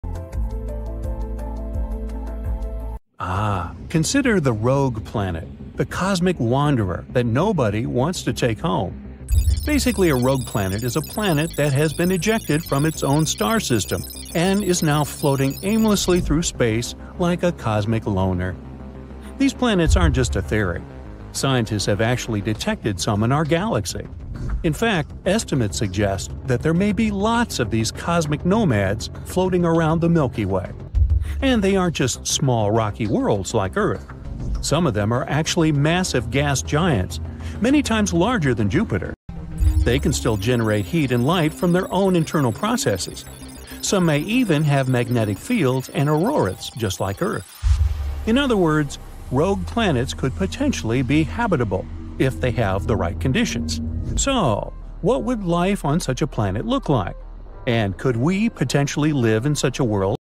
Lost in Space: Rogue Planets – The Galaxy's Mysterious Nomads!
Uncover the mysteries of rogue planets, cosmic wanderers ejected from star systems! We explore their detection, nature, and surprising characteristics, including potential for heat, magnetic fields, and even habitability. Join our journey through space to discover these cosmic nomads!